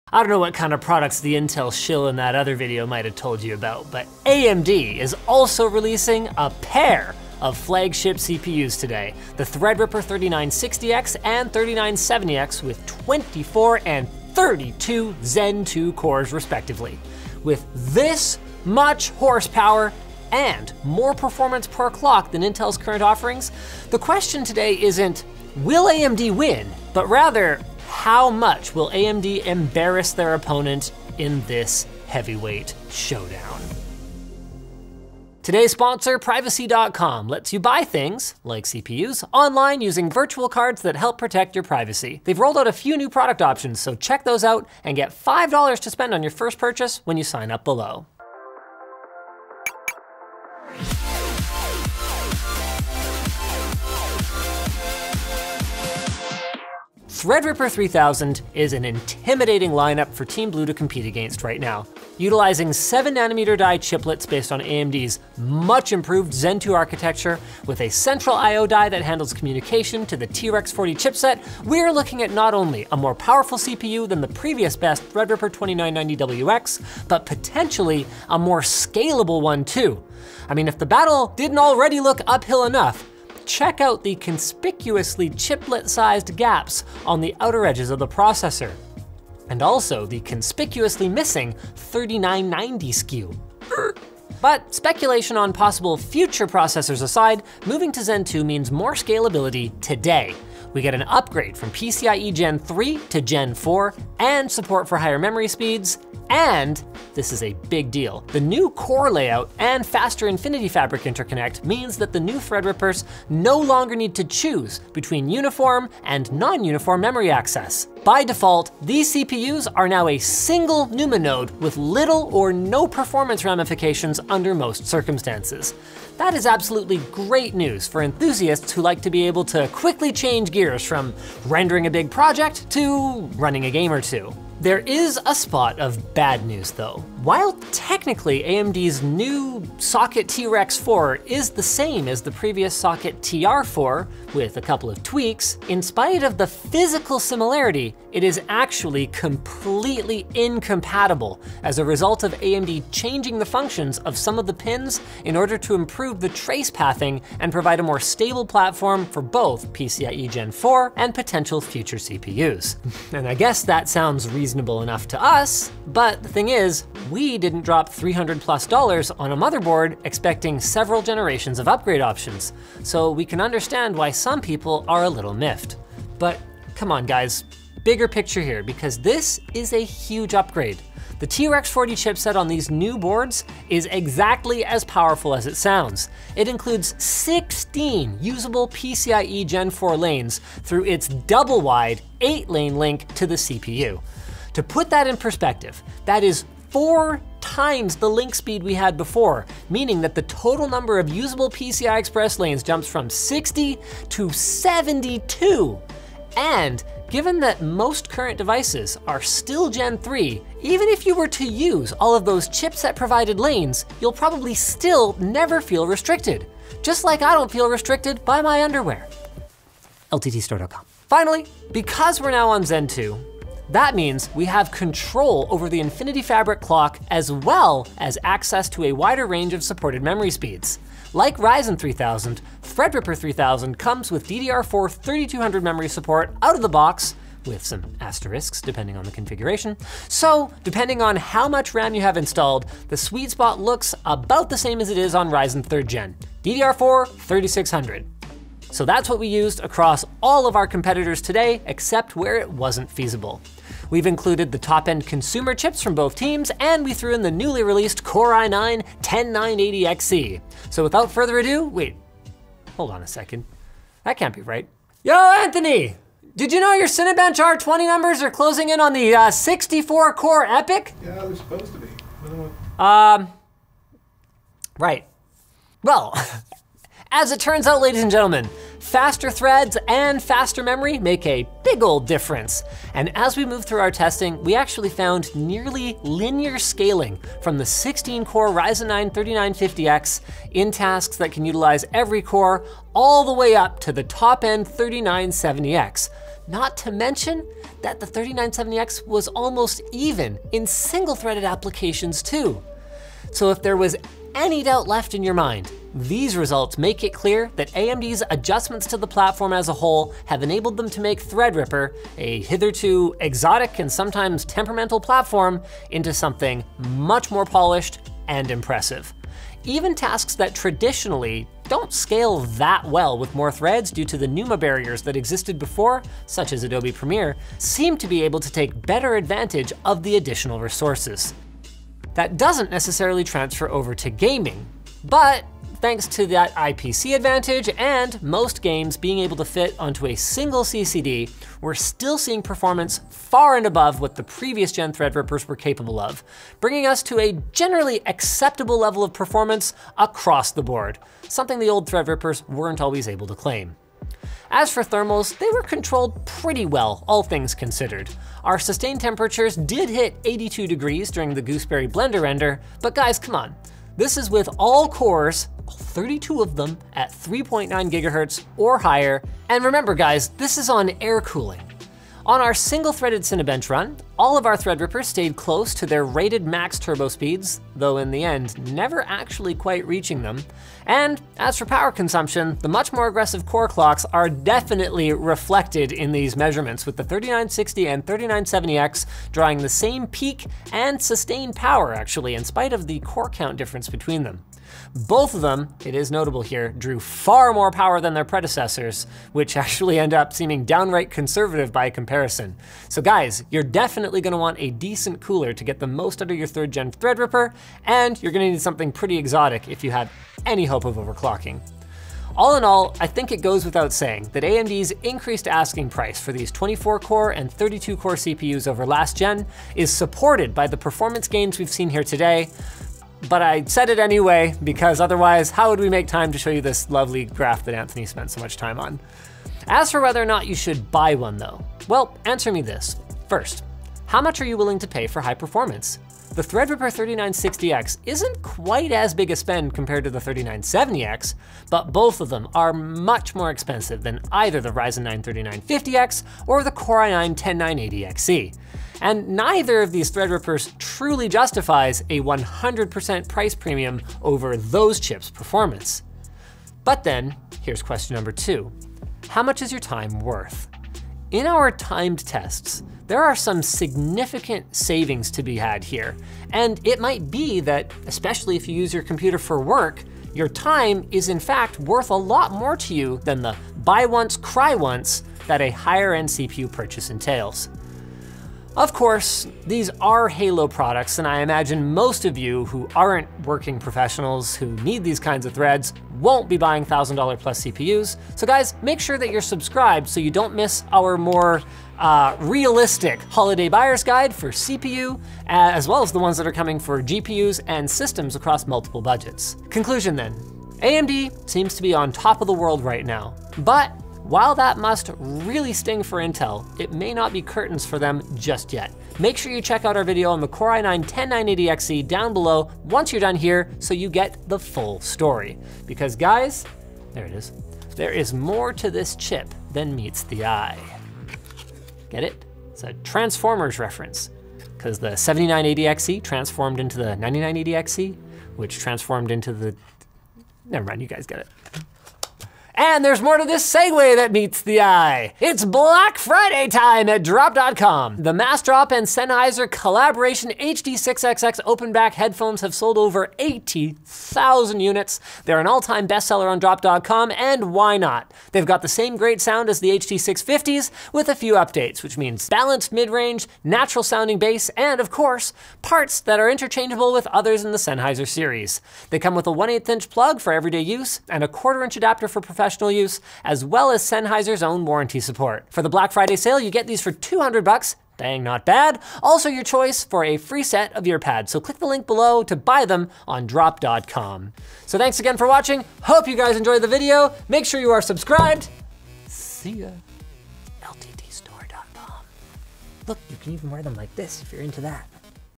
AMD clearly isn’t tired of winning yet… - Threadripper 3970X/3960X Review
​to get $5 off your first purchase!

After the Ryzen 9 3950X wowed us, AMD’s dropping yet another beastly CPU: The 32-core Threadripper 3970X and 24-core 3960X. Let’s see just how well they rip threads…

Buy a Threadripper 3970X:
On Amazon (PAID LINK): TBD

On Newegg (PAID LINK)